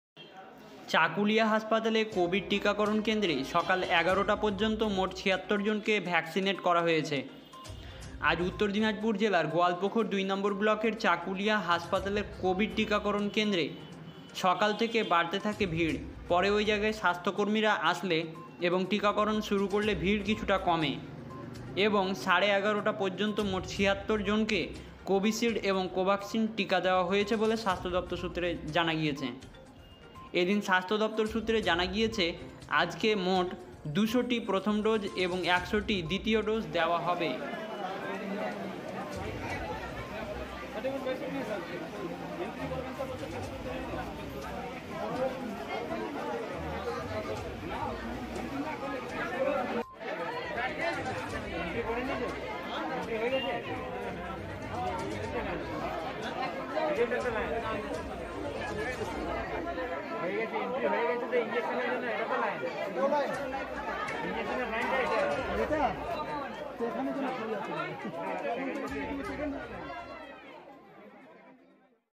চাকুলিয়া কোভিড টিকাকরণ কেন্দ্রে সকাল সাড়ে ১১ টা পর্যন্ত মোট ৭৬ জনকে ভ্যাকসিনেট করা হয়েছে।
চাকুলিয়া হাসপাতালে কোভিড টিকাকরণ কেন্দ্রে সকাল সাড়ে ১১ টা পর্যন্ত মোট ৭৬ জনকে ভ্যাকসিনেট করা হয়েছে।

আজ উত্তর দিনাজপুর জেলার গোয়ালপোখর 2 নম্বর ব্লকের চাকুলিয়া হাসপাতালে কোভিদ টিকাকরণ কেন্দ্রে সকাল থেকে বাড়তে থাকে ভিড়, পরে ওই জায়গায় স্বাস্থ্যকর্মীরা আসলে এবং টিকাকরণ শুরু করলে ভিড় কিছুটা কমে এবং সাড়ে এগারোটা পর্যন্ত মোট 76 জন কে Covi shild এবং  covacxin টিকা দেওয়া হয়েছে বলে স্বাস্থ্য দপ্তর সূত্রে খবর।

এদিন স্বাস্থ্য দপ্তর সূত্রে জানা গিয়েছে আজকে মোট 200 টি প্রথম দোষ দেওয়া হবে এবং 100 টি দ্বিতীয় দজ দেওয়া হবে। মোট ৩০০ জনকে ভ্যাকসিনেট করা হবে